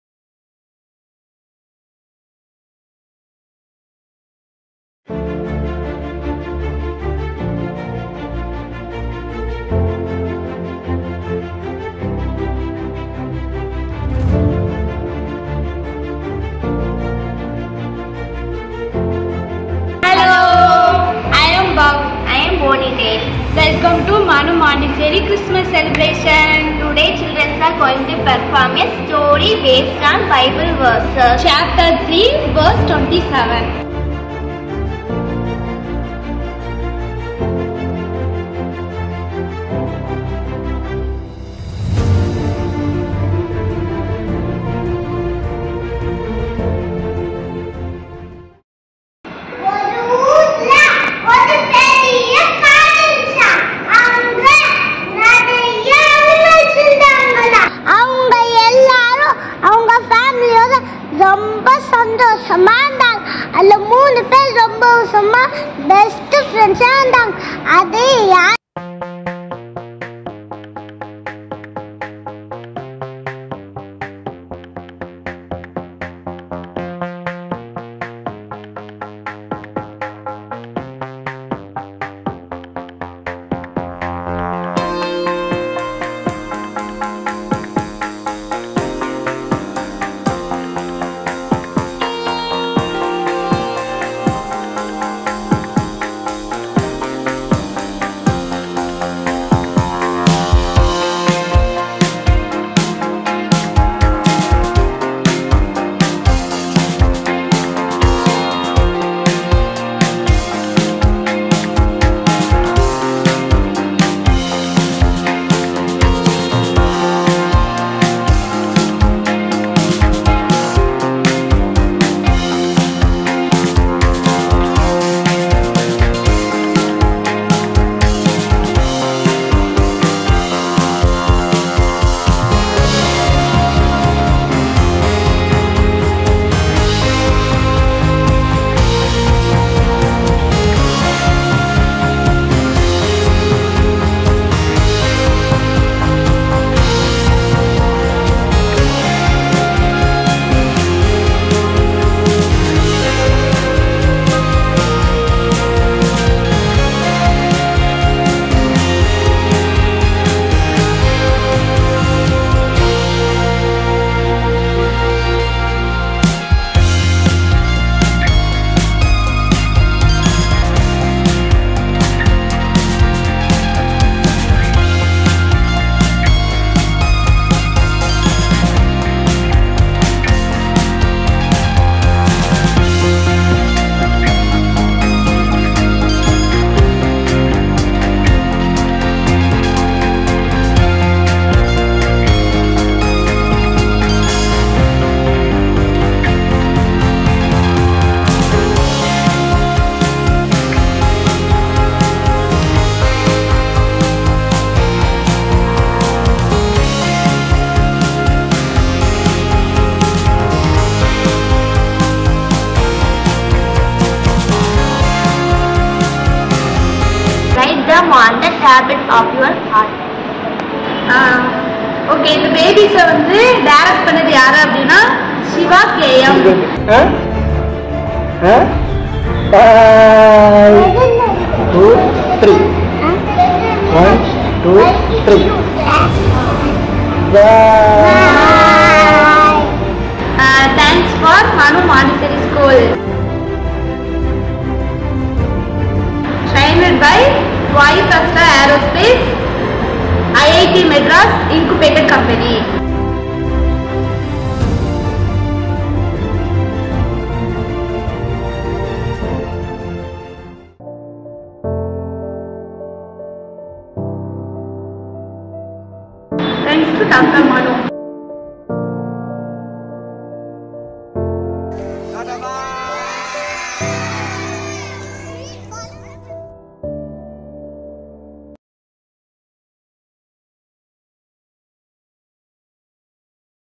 Dr Manu's Foundation Montessori and Nursery School | Christmas Day Celebration | Vaayusastra Theatre
Dr Manu's Foundation Montessori and Nursery School | Christmas Day Celebration | Vaayusastra Puppet and Theatre workshop for children

Vaayusastra Children's Theatre Program
(Celebration Series - Part 1)

Story - Help your friends
(Based on Bible verse 3:27)

Performed by - Krish Alan, Mariyam

Trainer - Shiva K M, Santhiya, Sindhiya

Venue - Auditorium, Dr.Manu's foundation,
             Montessori Nursery school

Puppet & Theatre Program
by

RTBI, IIT Madras Research park,
Chennai - 600 013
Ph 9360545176